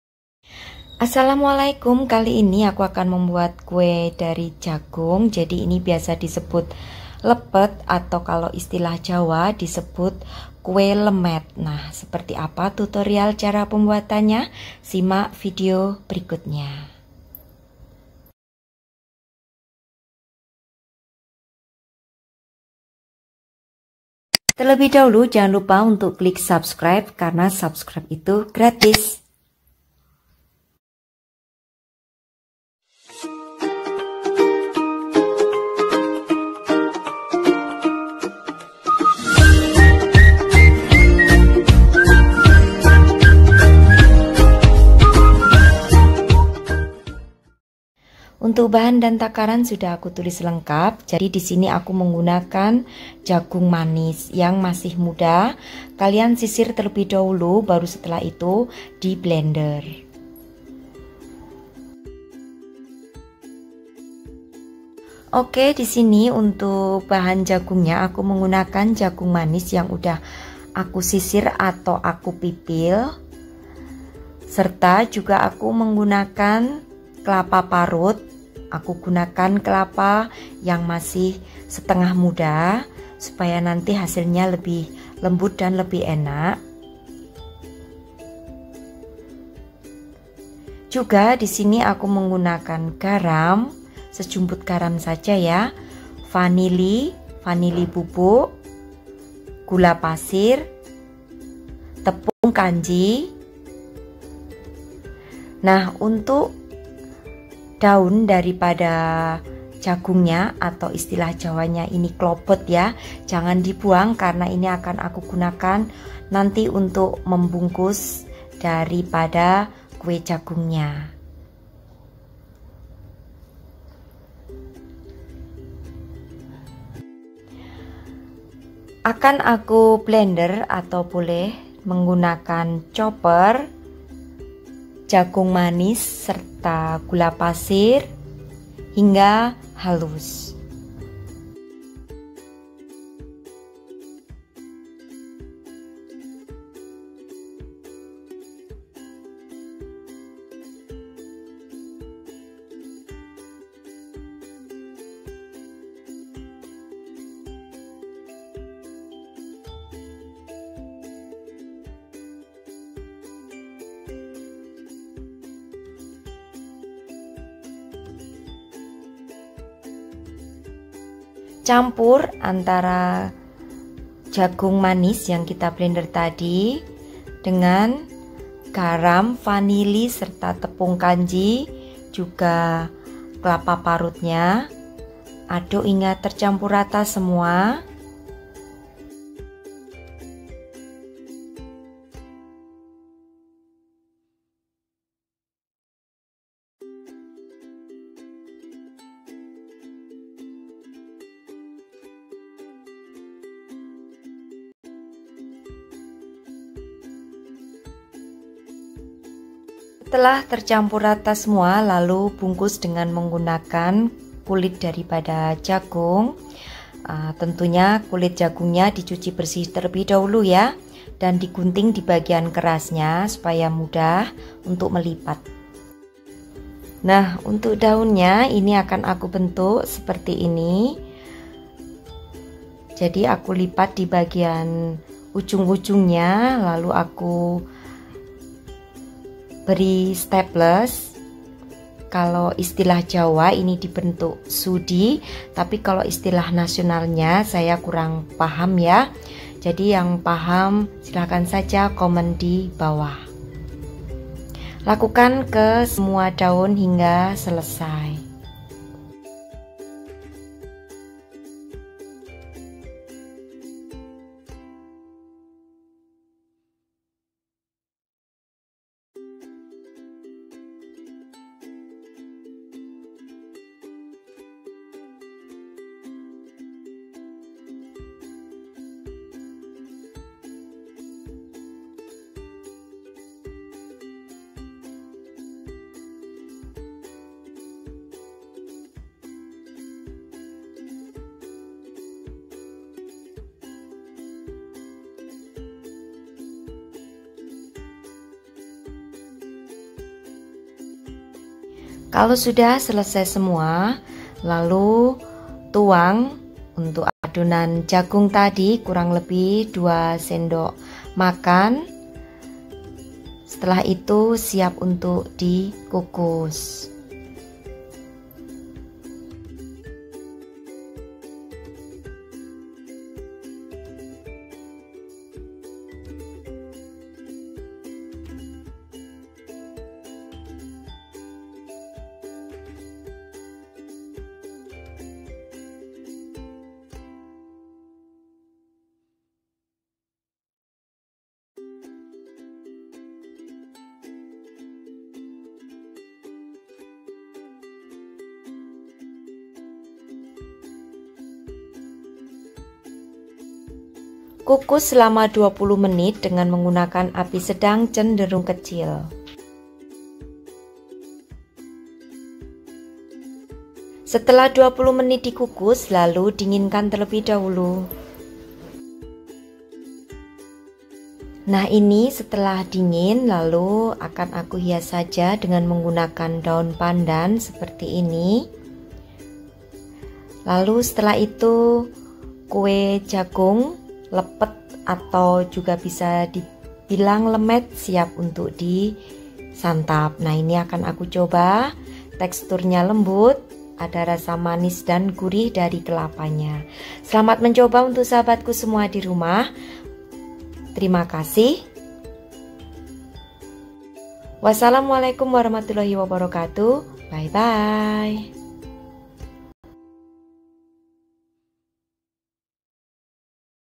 Resep kue jagung/ lepet/ lemet
bahan
2 buah jagung
2 sdm gula pasir
5 sdm kelapa parut
4 sdm tepung kanji
sejumput garam
vanili